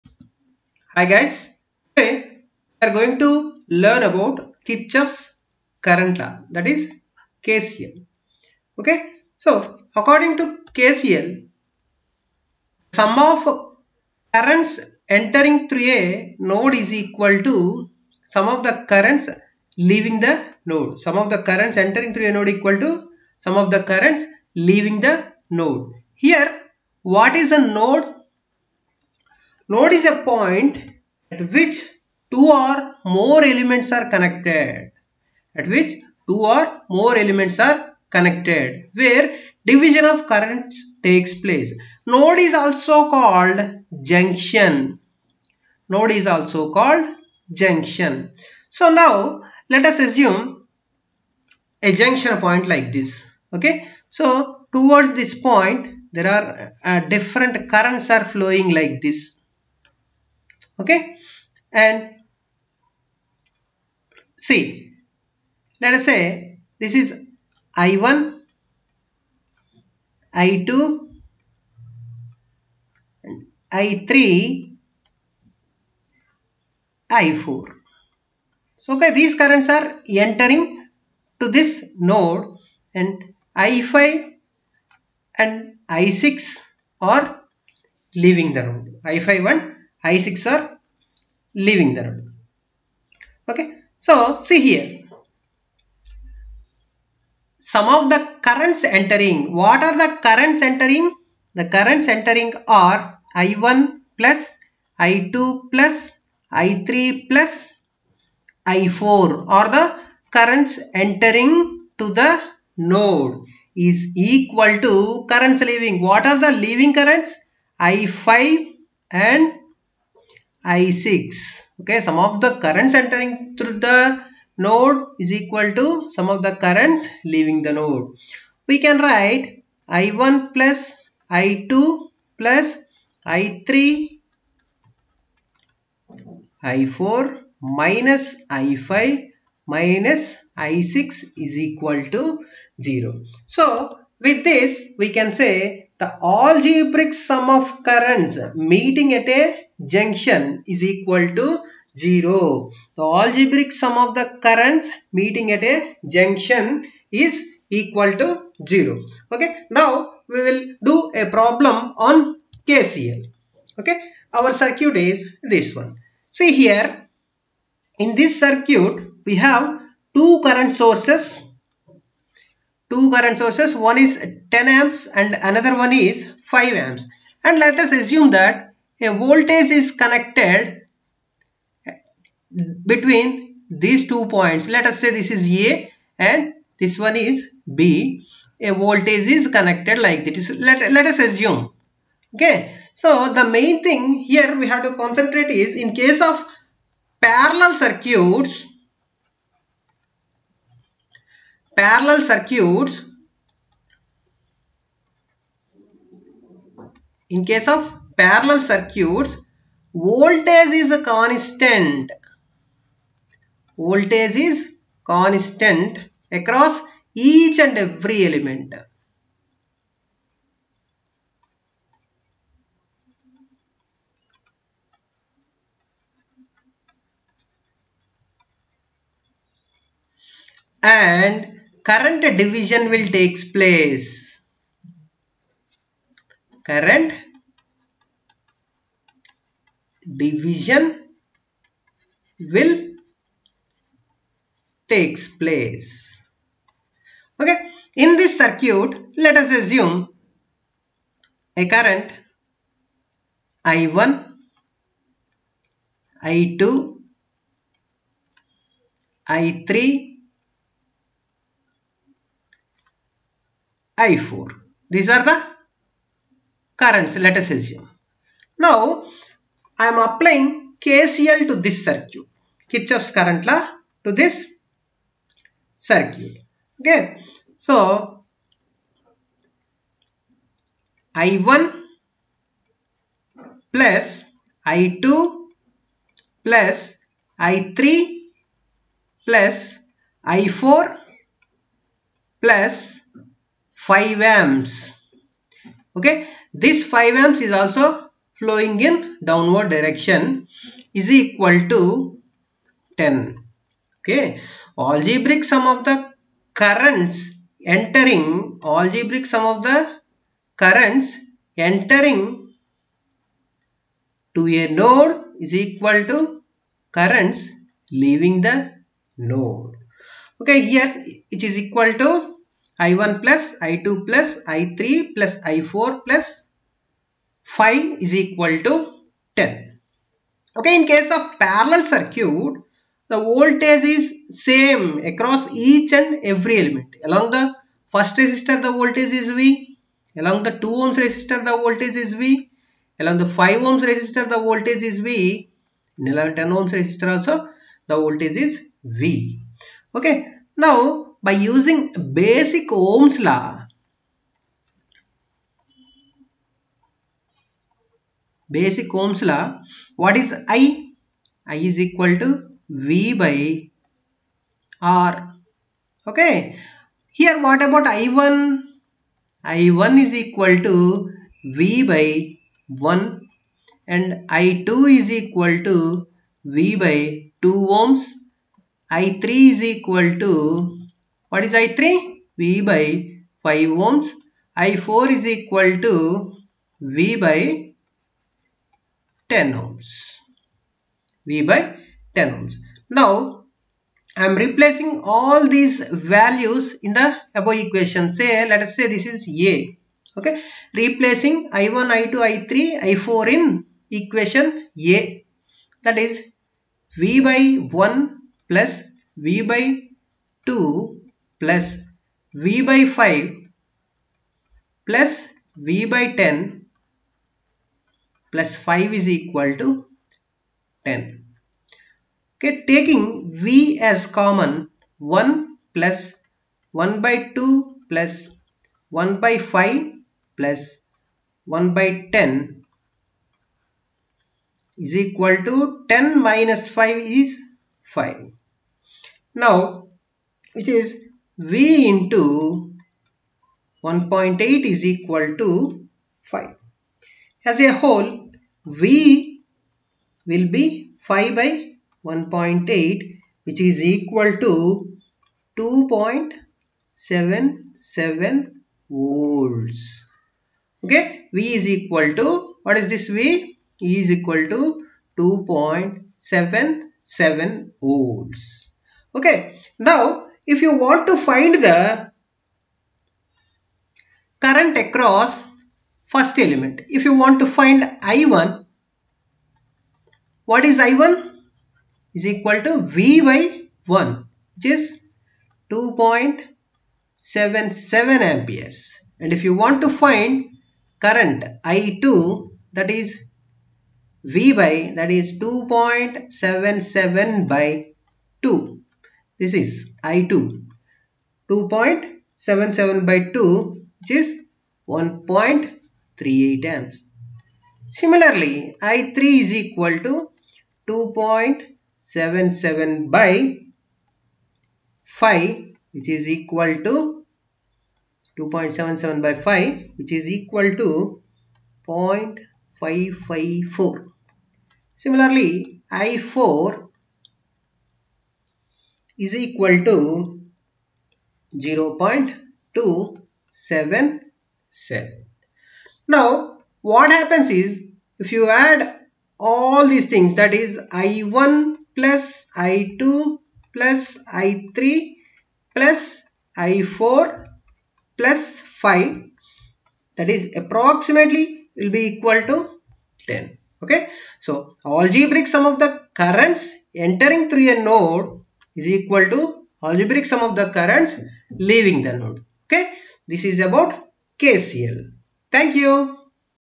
Kirchhoff's Current Law | KCL | Electrical Circuits | Lec-12
Electrical Circuits
Kirchhoff's Current Law (KCL) explained with resistive circuit